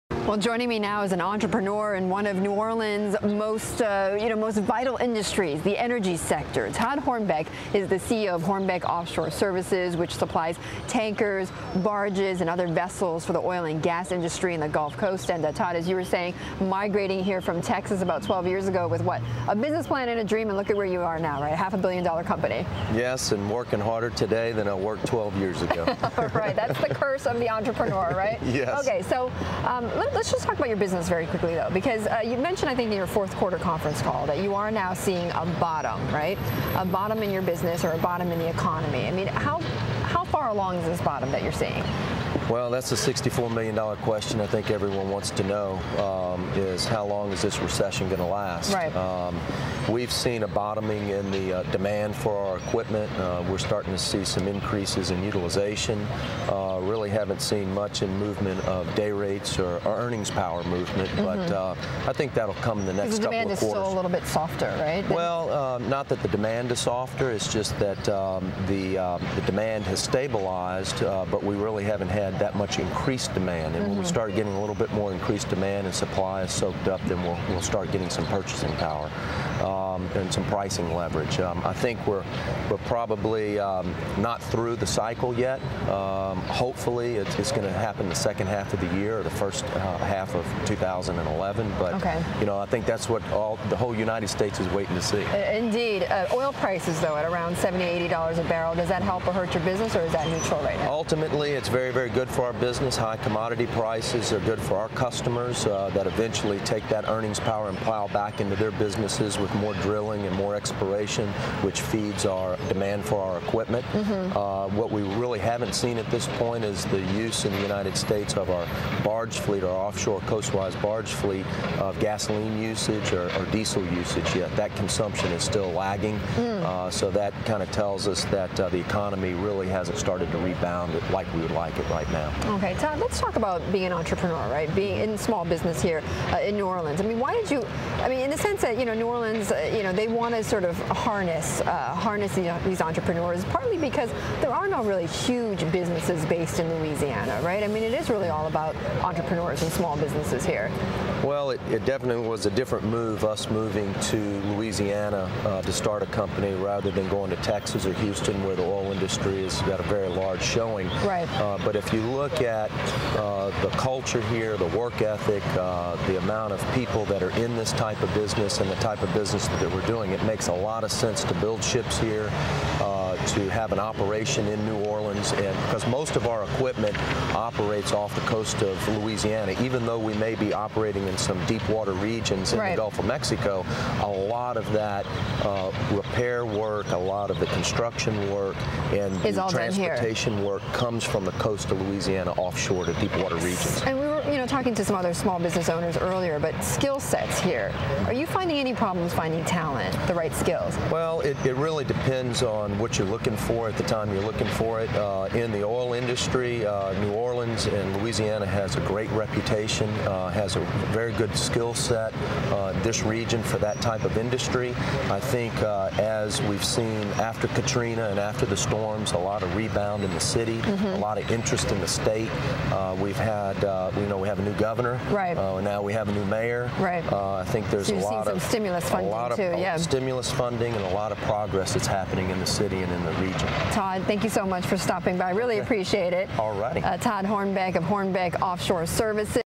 Hornbeck Offshore's Hornbeck Sees Demand Bottoming: Video
March 23 (Bloomberg) -- Todd Hornbeck, president and chief executive officer of Hornbeck Offshore Services Inc., talks with Bloombergs Betty Liu about the outlook for the oil and gas industry in Louisiana. (Source: Bloomberg)